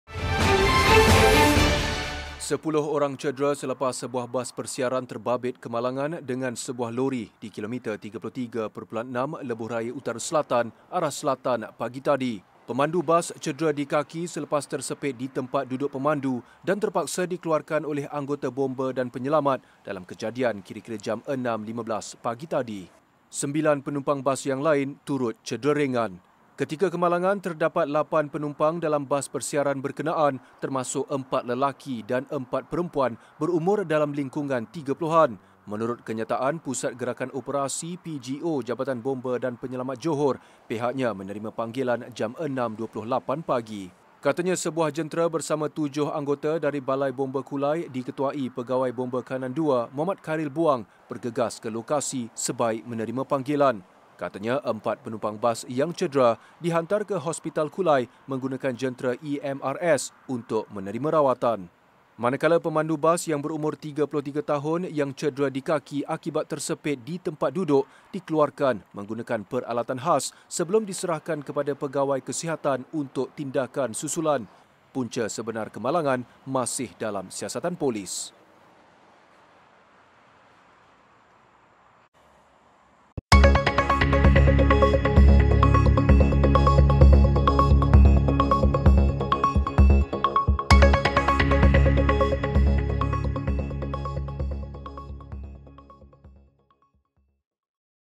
Pemandu Bas Tersepit, 9 Lagi Cedera
10 orang cedera selepas sebuah bas persiaran terbabit kemalangan dengan sebuah lori di kilometer 33.6, Lebuhraya Utara-Selatan, arah selatan, pagi tadi.